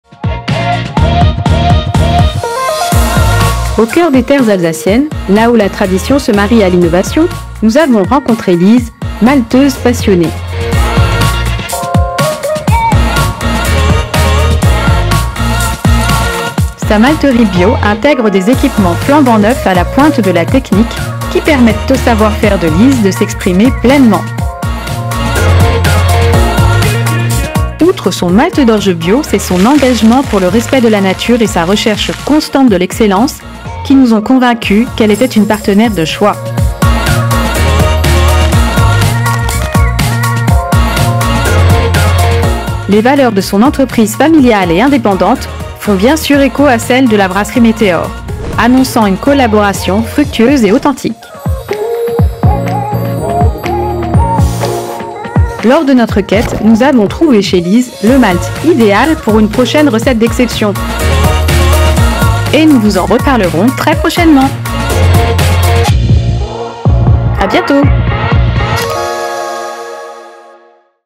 Bière éphémère Meteor x Maltala
Aujourd’hui, on vous emmène à la rencontre de Maltala, la malterie artisanale alsacienne fondée par Lise Meinrad. Dès notre première rencontre, nous avons su que nos valeurs communes allaient donner naissance à quelque chose de grand. 🌟
Comme chez METEOR, chez Maltala, c’est une affaire de famille ! Ensemble, nous avons exploré le savoir-faire du maltage pour créer une bière éphémère qui rend hommage à nos racines alsaciennes. 🎥